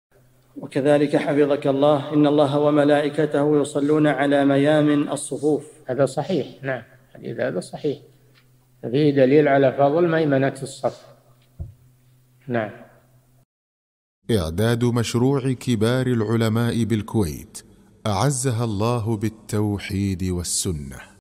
[127 -582] ما صحة حديث: (إن الله وملائكته يصلون على ميامن الصفوف)؟ - الشيخ صالح الفوزان
-"السؤال: وكذلك حفظك الله (إن الله وملائكته يصلون على ميامن الصفوف)
الجَوَابُ: هذا صحيح. نعم، الحديث هذا صحيح وفيه دليل على فضل ميمنة الصف.
للاشتراك في قناة الشيخ صالح الفوزان
سؤال وجواب من شرح صفة الصلاة ( 582 ) مقطع

للاستفسار والإقتراحات والدعم
رئيس الفريق
لمشروع كبار العلماء